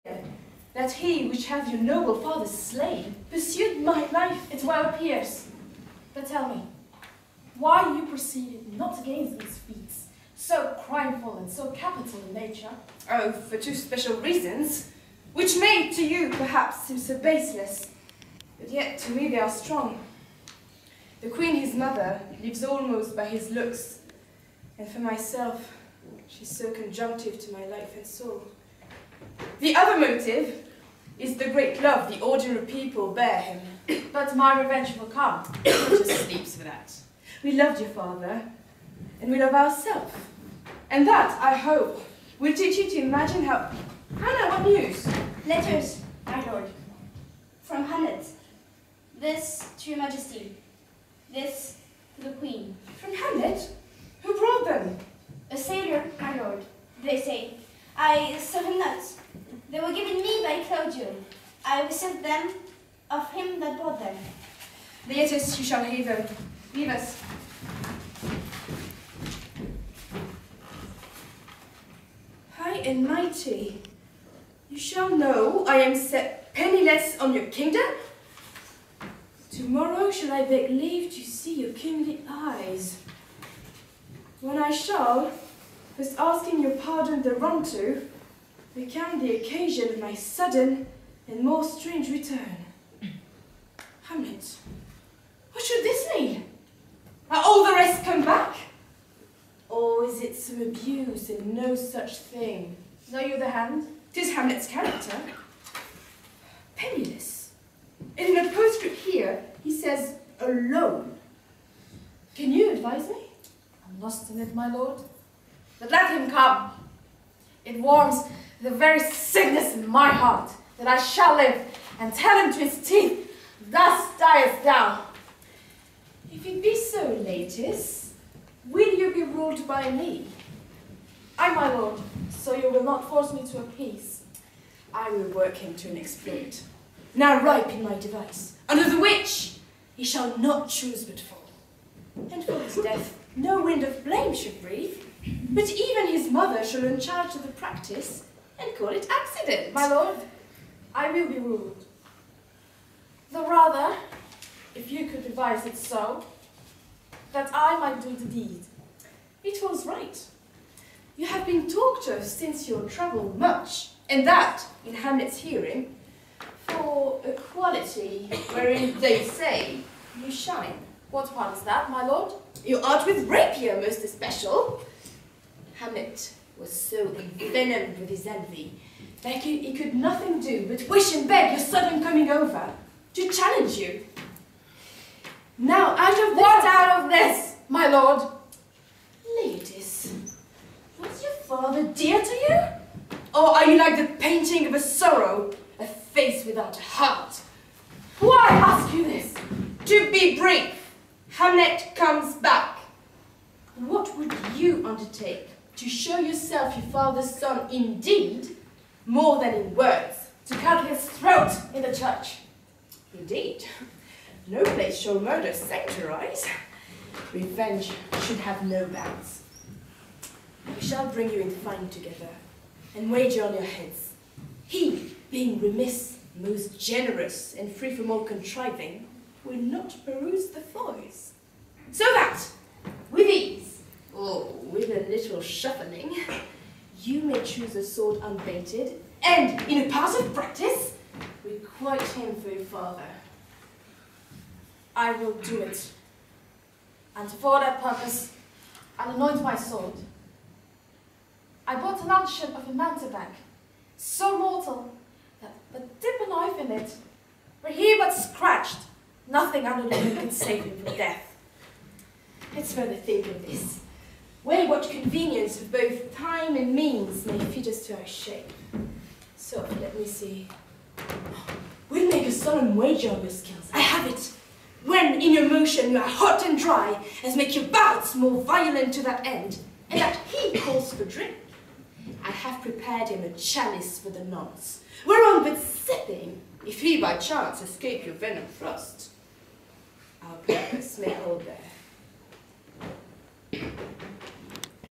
BELT (Besançon English Language Theater): Hamlet - A Plot to Kill Hamlet
Played on 11 April 2013
Théâtre Bacchus, Besançon, France